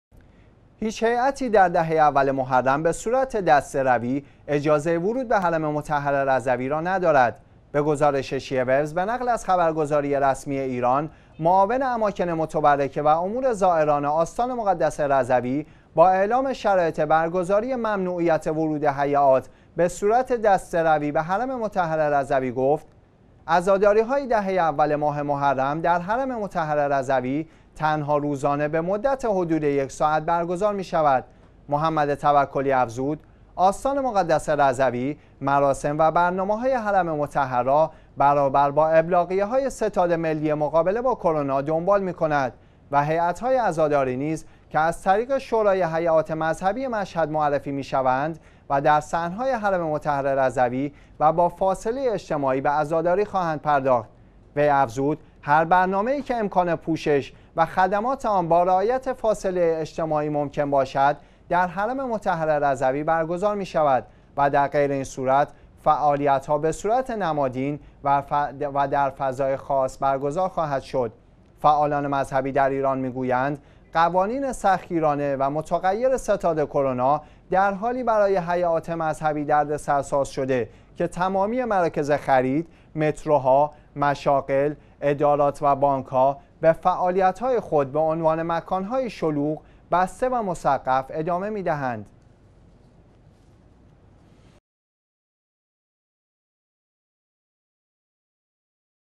ممنوعیت ورود دسته های عزاداری به آستان مقدس رضوی
هیچ هیئتی در دهه اول محرم به صورت دسته‌روی اجازه ورود به حرم مطهر رضوی را ندارد.
به گزارش شیعه ویوز به نقل از خبرگزاری رسمی ایران، معاون اماکن متبرکه و امور زائران آستان مقدس رضوی با اعلام شرایط برگزاری ممنوعیت ورود هیئات به صورت دسته‌روی به حرم مطهر رضوی گفت: عزاداری‌های دهه اول ماه محرم در حرم مطهر رضوی تنها روزانه به مدت حدود یک ساعت برگزار می‌شود.
«محمد توکلی» افزود: آستان مقدس رضوی مراسم و برنامه های حرم مطهر را برابر با ابلاغیه‌های ستاد ملی مقابله با کرونا دنبال می‌کند و هیئت های عزاداری نیز که از طریق شورای هیئات مذهبی مشهد معرفی می‌شوند و در صحن‌های حرم مطهر رضوی و با فاصله اجتماعی به عزاداری خواهند پرداخت.
وی افزود: هر برنامه‌ای که امکان پوشش و خدمات آن با رعایت فاصله اجتماعی ممکن باشد در حرم مطهر رضوی برگزار می‌شود در غیر این صورت فعالیت‌ها به صورت نمادین و در فضایی خاص برگزار خواهد شد.
فعالان مذهبی در ایران می گویند، قوانین سختگیرانه و متغییر ستاد کرونا در حالی برای هیئات مذهبی دردسر ساز شده که تمامی مراکز خرید، متروها، مشاغل، ادارات و بانک ها به فعالیت های خود به عنوان مکان هایی شلوغ، بسته و مسقف ادامه می دهند.